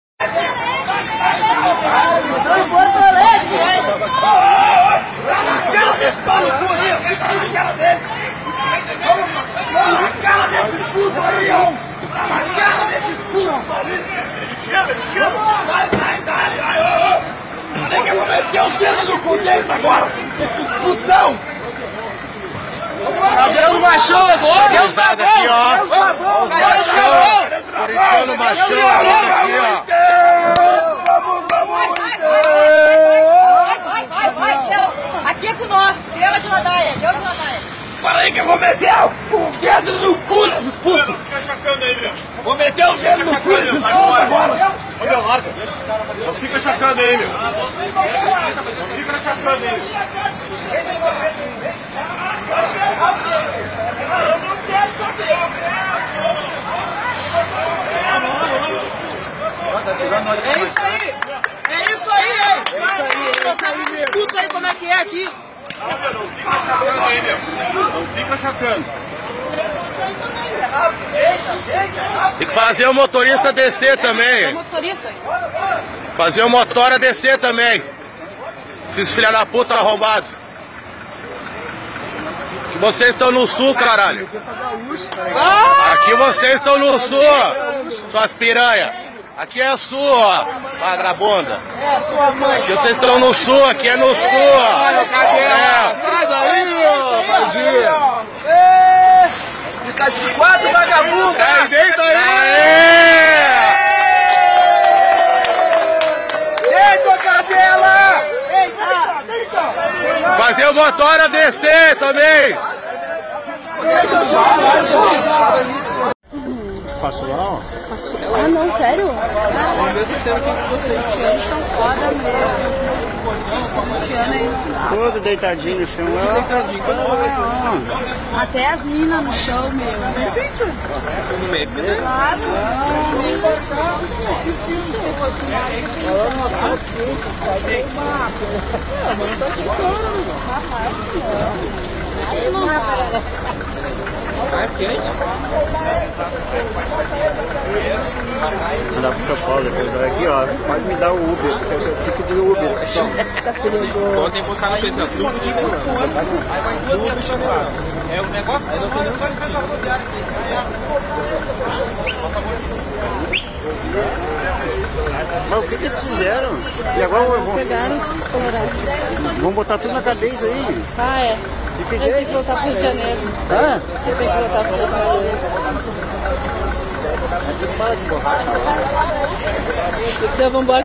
Polícia gaúcha humilha corinthianas e colorados comemoram
Em Porto Alegre, a Polícia Militar mandou torcedores do Corinthians descerem do ônibus e deitarem de rostos colados no chão enquanto torcedores do Inter